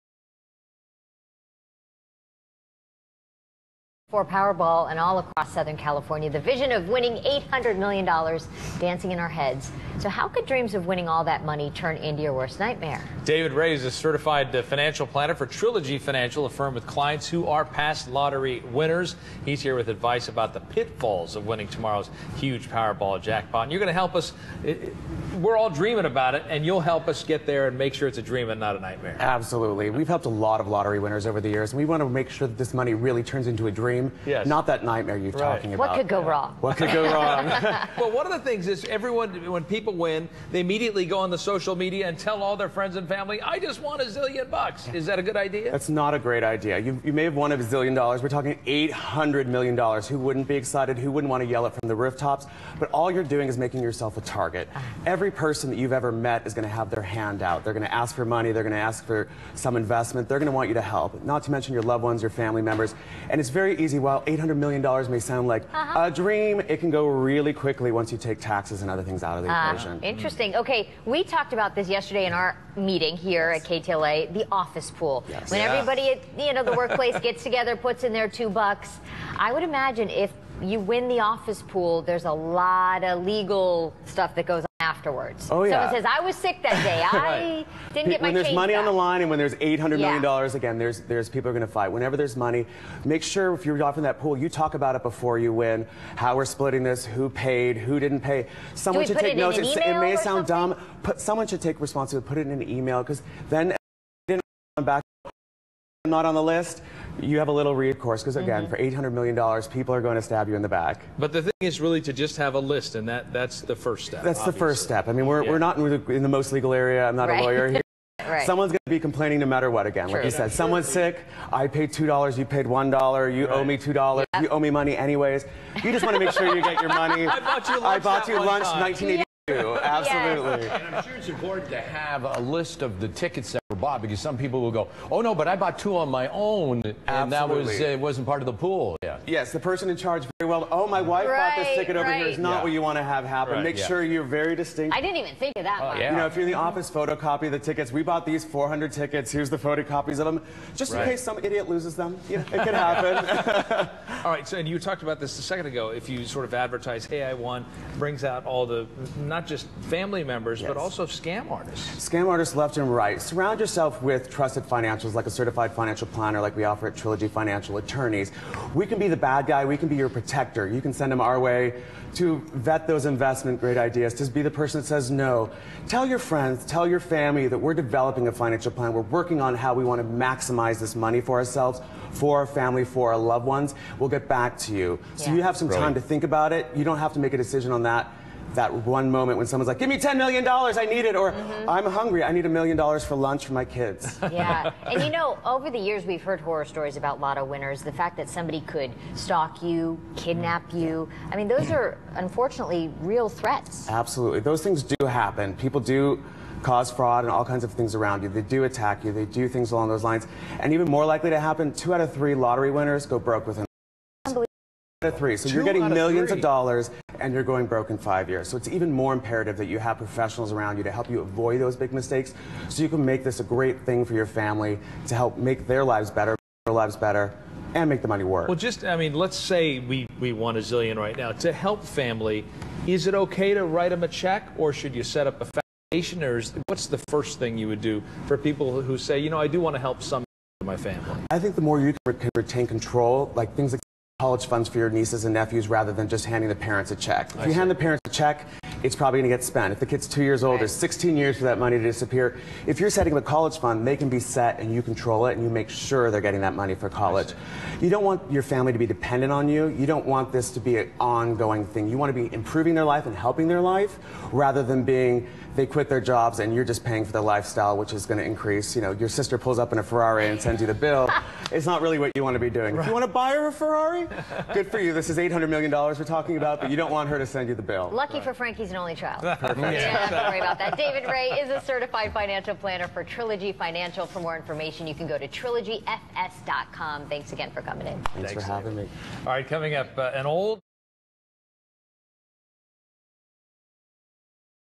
CFP David Rae on KTLA 5
CFP for Trilogy Financial David Rae, offers advice about what financial and legal steps to take when playing/winning the lotto on KTLA 5.